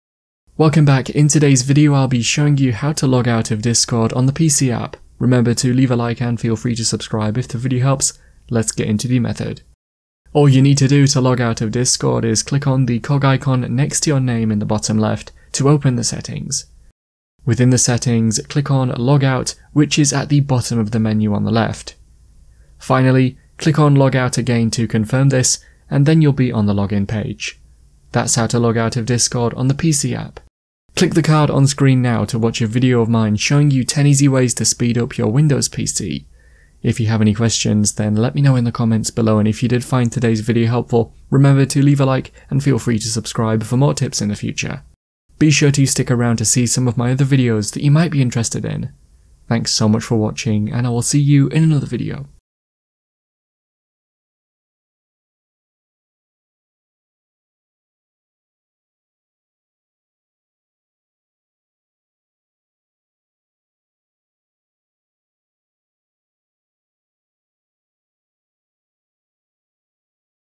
How to Log Out of Discord on PC Windows 10/11
Want to know how to log out of Discord on PC in Windows 10 or Windows 11? This video will show you how to logout of Discord on PC. You might want to know how to logout of Discord account on PC to log in to a new one or to keep your account secure. Hopefully today’s video helps show you how to log out of your discord account on PC in Windows 10 or 11!